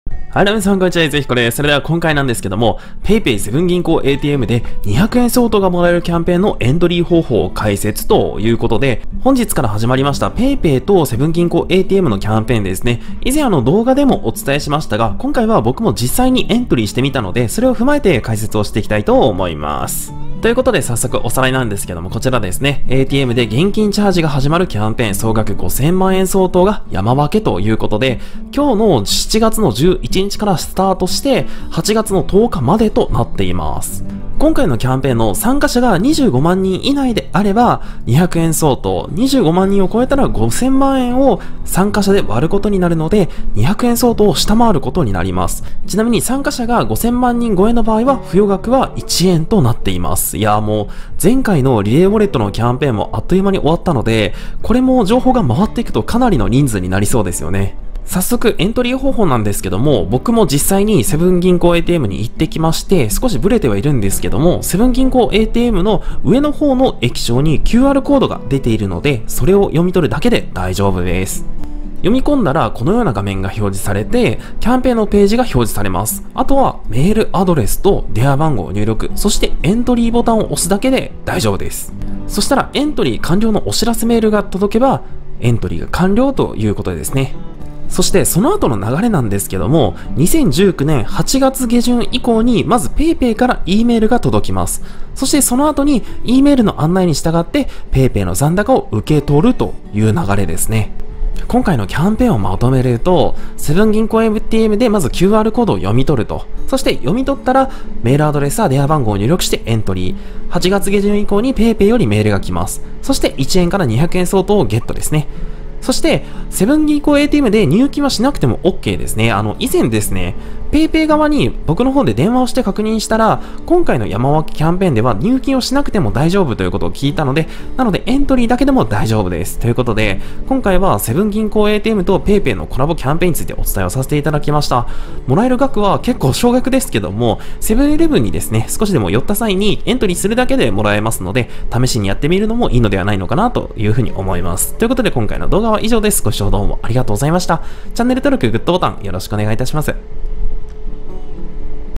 PayPay5000万円山分けキャンペーン！PayPay×セブン銀行ATMで200円相当がもらえるエントリー方法を解説
【PR】 PayPay
キャンペーンのページ

使用機材
マイク：Rode NT1A
キャプチャーボード：I-O DATA GV-USB3
オーディオインターフェース：YAMAHA AG03